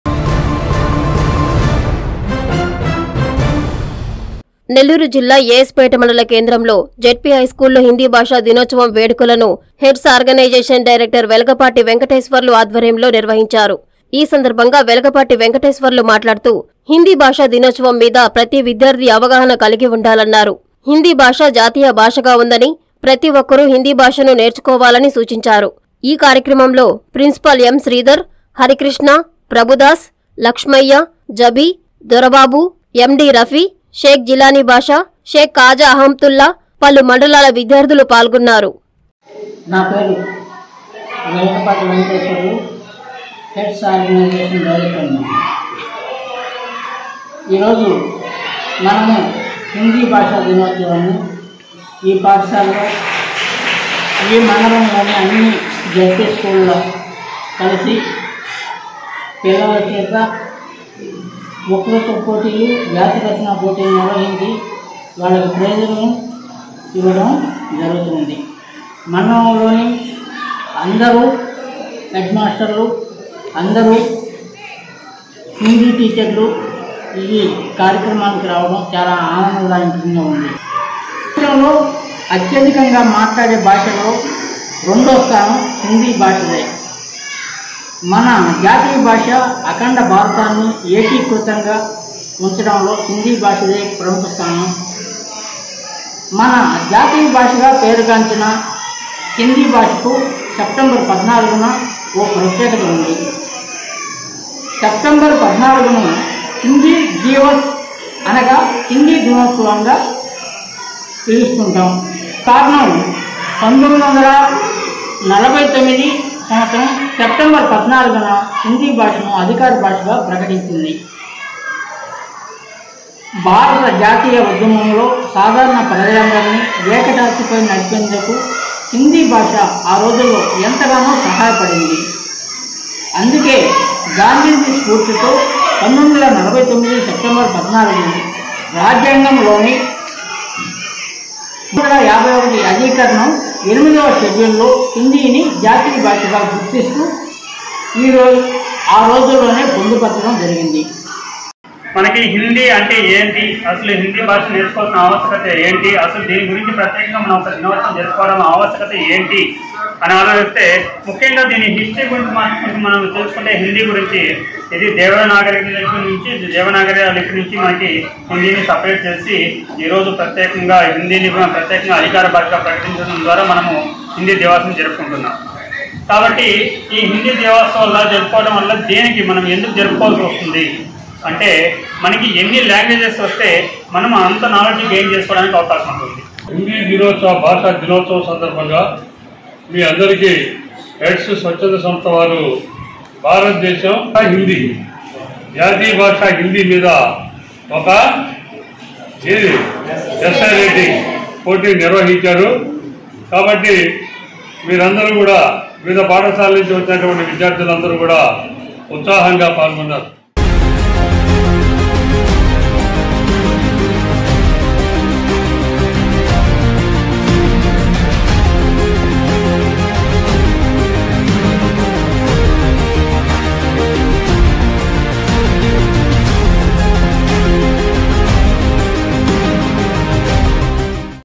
ప్ర‌తీ ఒక్క‌రూ హిందీ భాష నేర్చుకోవాలి | Hindi Diwas | AS Peta
- ఏఎస్‌పేట జ‌డ్పీ హైస్కూల్లో హిందీ భాషాదినోత్స‌వం
- హెడ్స్ ఆర్గ‌నైజేష‌న్ డైరెక్ట‌ర్ వెల‌గ‌పాటి వెంక‌టేశ్వ‌ర్లు